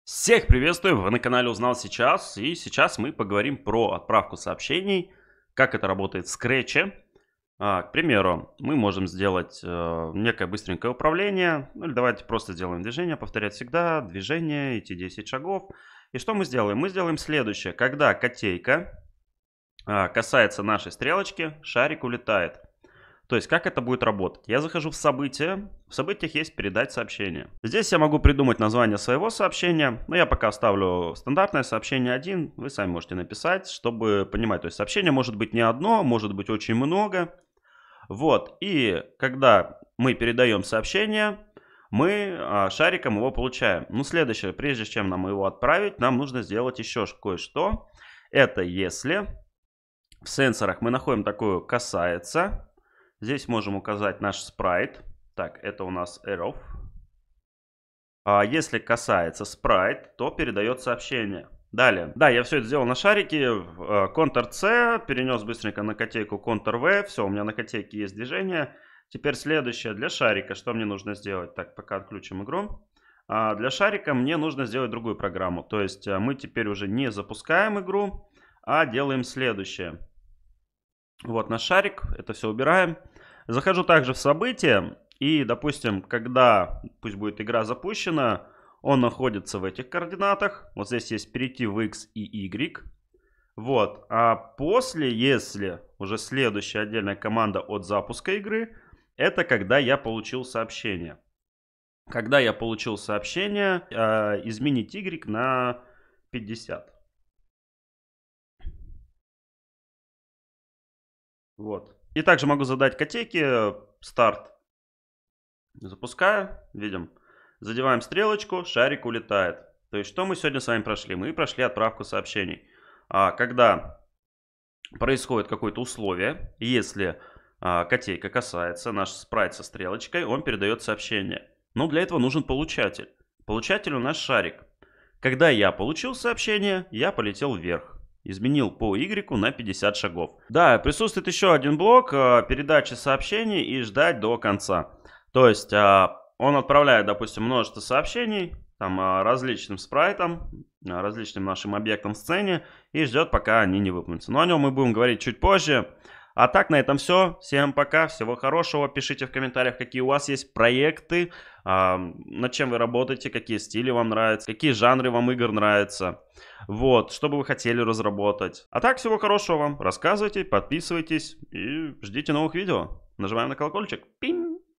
Как создать свою игру для начинающих| Отправить Сообщение| РАЗРАБОТЧИК ИГР! SCRATCH 1lvl + 800XP
Этот курс обучит вас разрабатывать свои игры и анимации в очень простой но в тоже время интересной программе Scratch.
Хотите разрабатывать свой игры ?
Как создать свою игру.
Знакомства со Scratch!

Шаг № 11  в SCRATCH 1 уровень + 800XP.
Создаем свой игры "Отправить Сообщение"

Всем Привет, меня зовут Никита Романович
Если у вас остались вопросы пишите:)